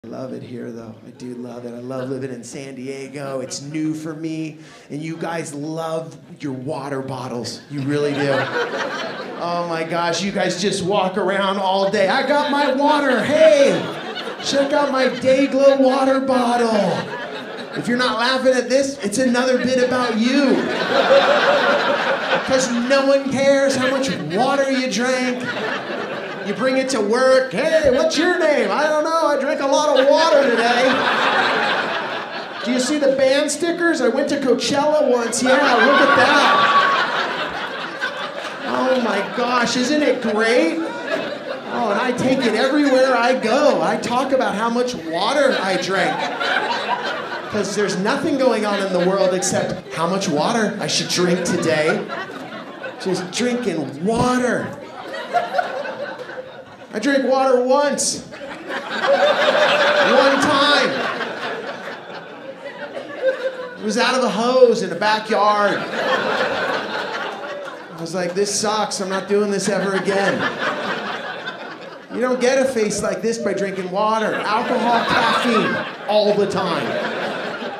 No One Cares About Your Water Bottle | Joe Sib
Live from the world famouse Comedy Store in La Jolla California.

for tour dates.